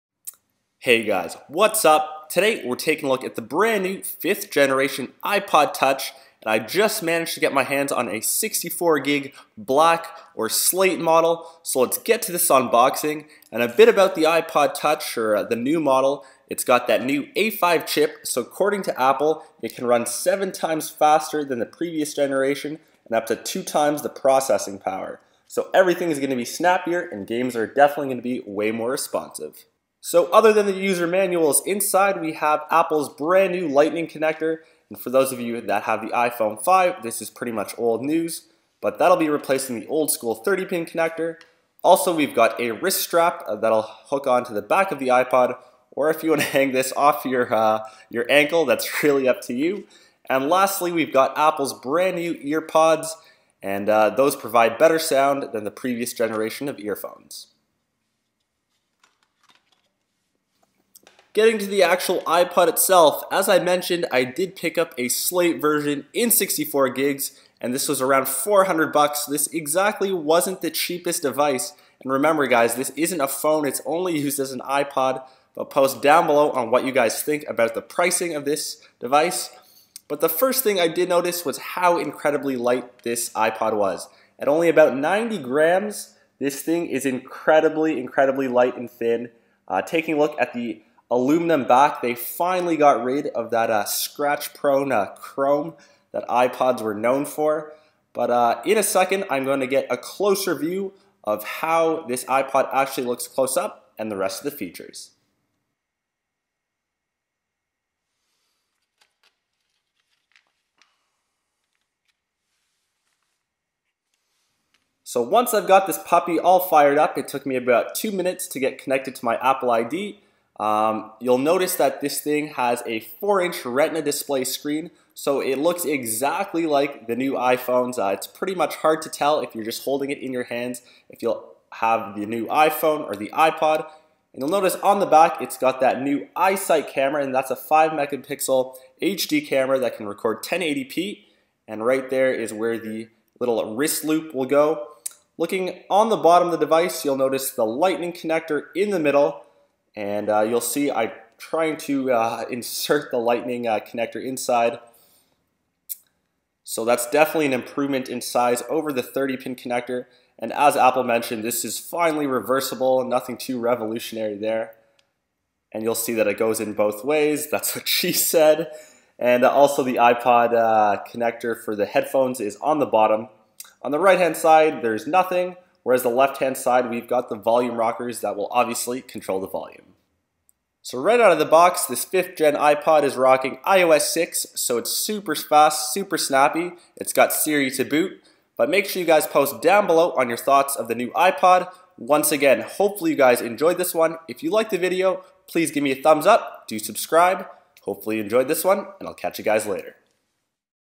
5th Generation iPod Touch Black/Slate 64GB
Taking a look at the brand new 64gb Black 5th Generation iPod Touch